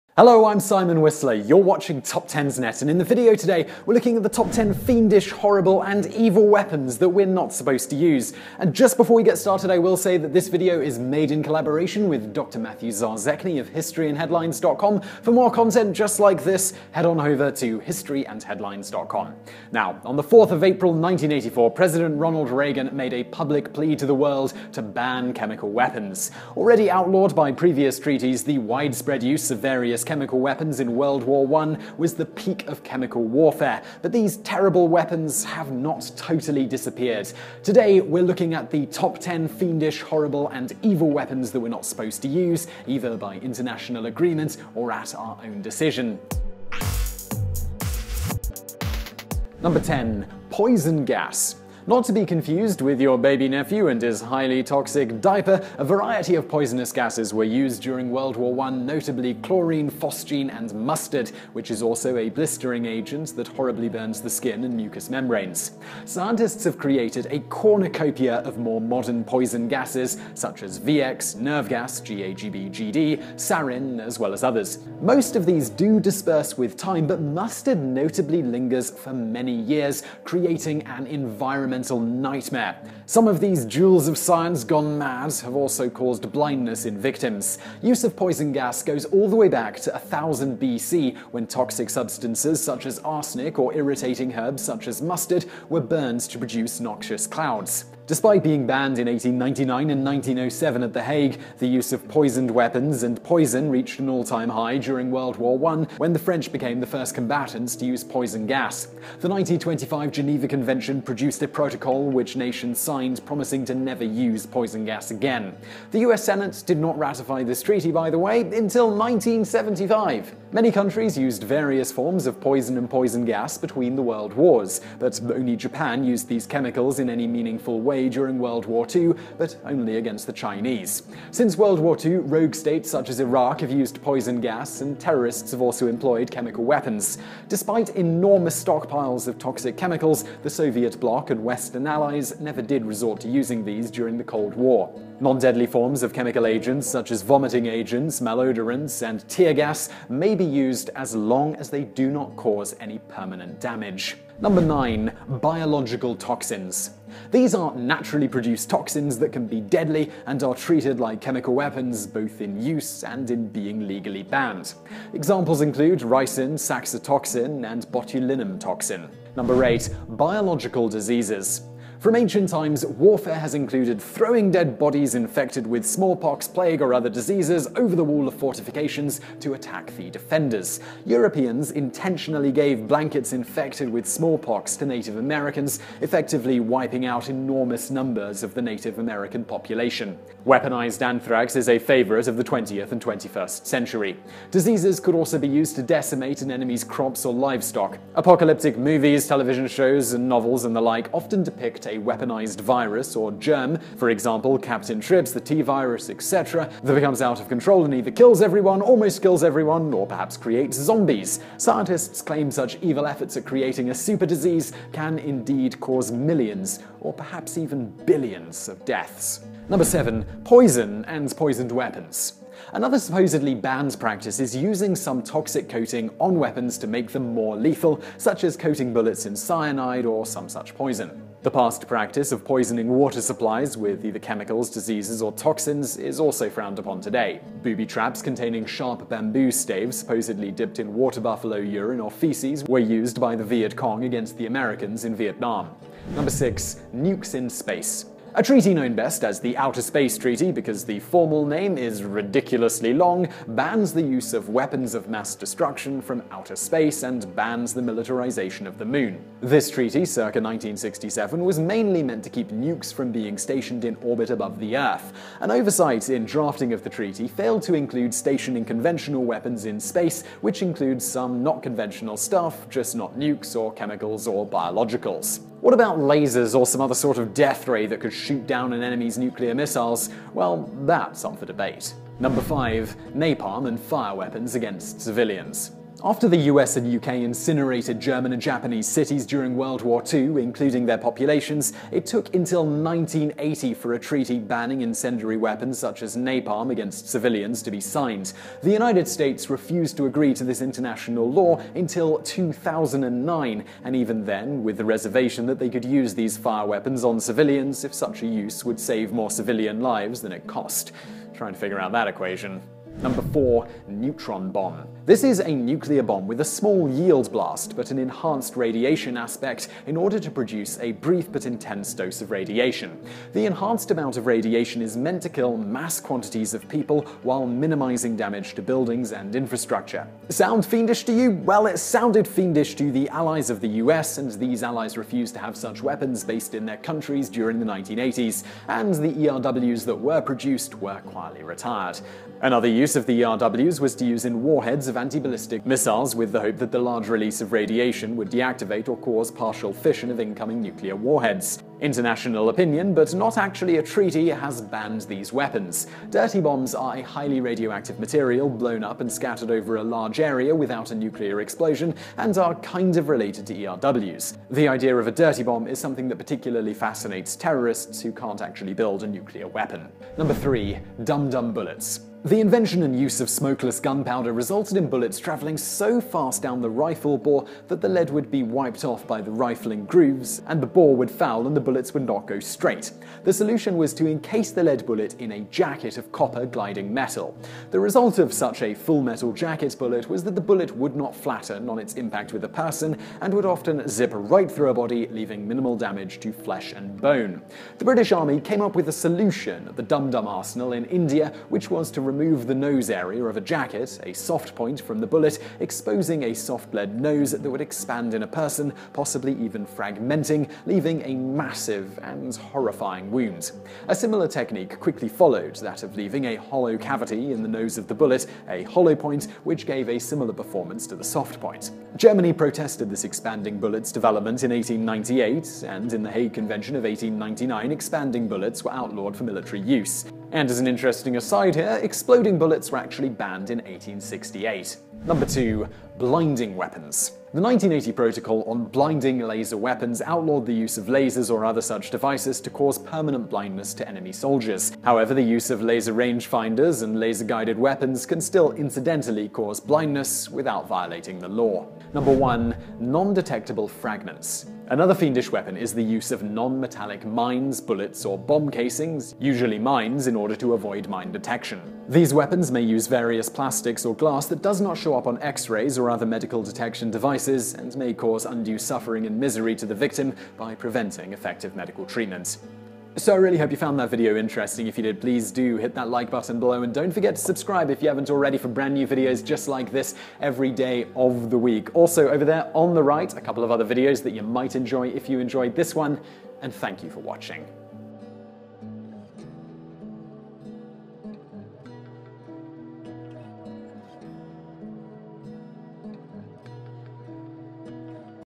Top 10 HORRIBLE and EVIL Weapons We are NOT Supposed to Use
On April 4, 1984, President Ronald Reagan made a public plea to the world to ban chemical weapons.  Already outlawed by previous treaties, the widespread use of various chemical weapons in World War I was the peak of chemical warfare, but these terrible weapons have not totally disappeared.  Today we list 10 Fiendish, Horrible, Evil Weapons We’re Not Supposed to Use, either by International agreement or our own decision.  At times even crossbows were banned for use against “Christians,” as were square projectiles.  What weapons, devices, or techniques from Hell would you include on a ban against such things?

Help us translate our videos - Learn more why you might want to help

Entertaining and educational top 10 lists from TopTenzNet!

Coming up:

10.  Poison Gas.
9.  Biological Toxins.
8.  Biological Diseases.
7.  Poison and Poisoned weapons.
6.  Nukes in Space.
5.  Napalm and Fire Weapons against Civilians.
4.  Neutron Bomb.
3.  Dum-Dum bullets.
2.  Blinding weapons.
1.  Non-detectable Fragments.